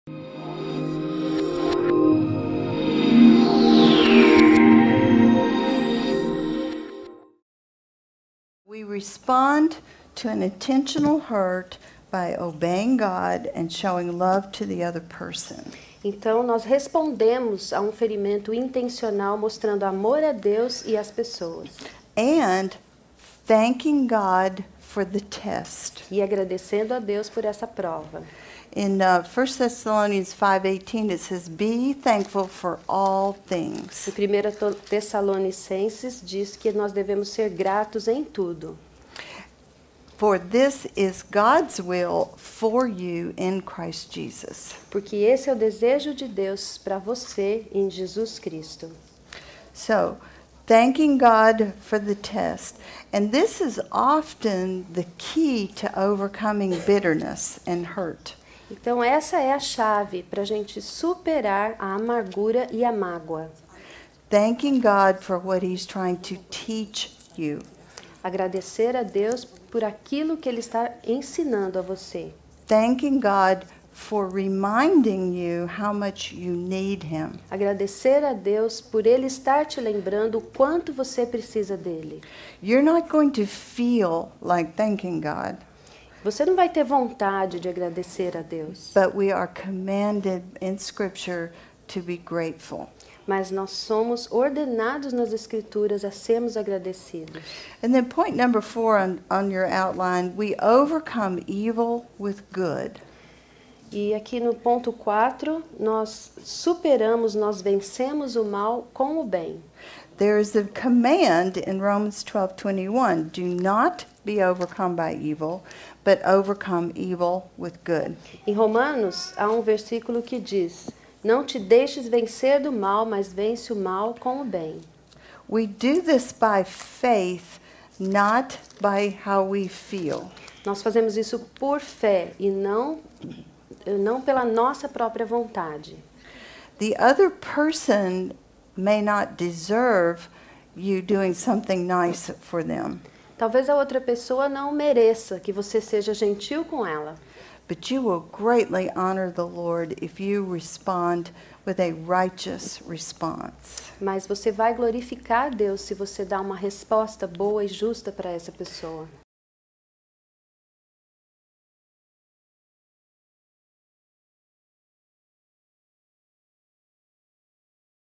Em Tudo Dai Graças - Martha Peace
Trecho da mensagem de Martha Peace no Encontro para Mulheres 2012.